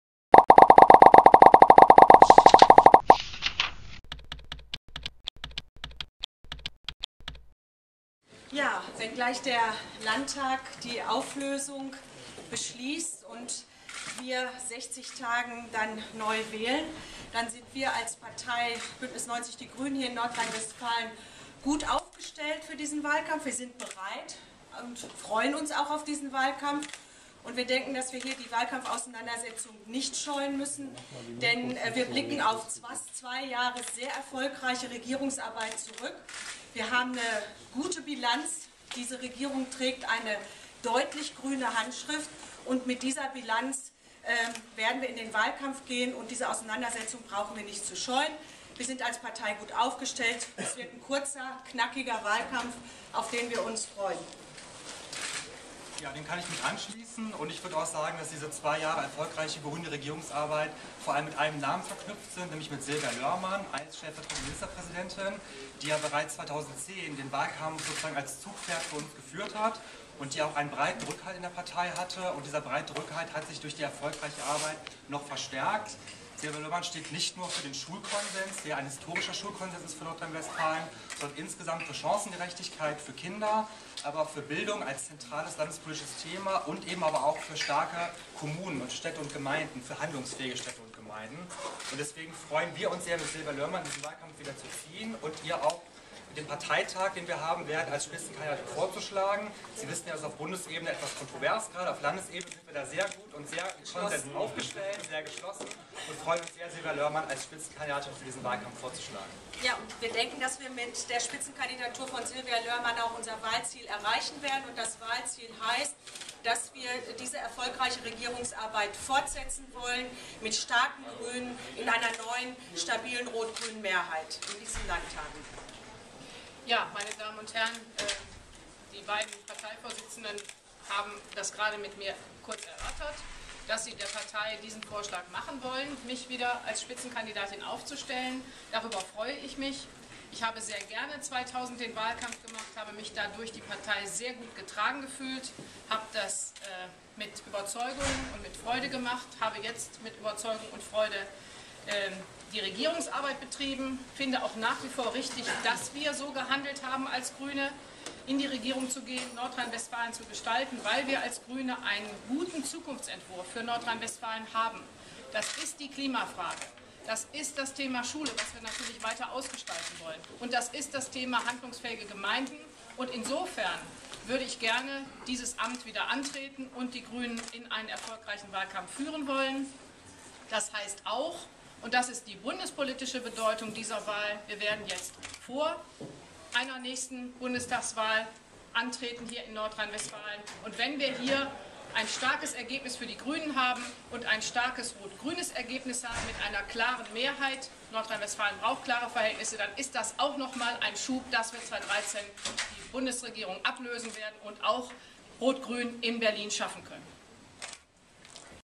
Erstes Statement: NRW vor Neuwahlen
Wir wollen weiterhin diese Verantwortung mit einer stabilen rot-grünen Mehrheit und noch stärkeren Grünen übernehmen. Dafür werden wir im Wahlkampf um Unterstützung werben. Wir werden euch für den Parteitag wieder Sylvia Löhrmann als unsere Spitzenkandidatin vorschlagen, die 2010 einen erfolgreichen Wahlkampf für uns bestritten und in den letzten zwei Jahren als stellvertretende Ministerpräsidentin und Schulministerin starke Akzente in der Landesregierung gesetzt hat.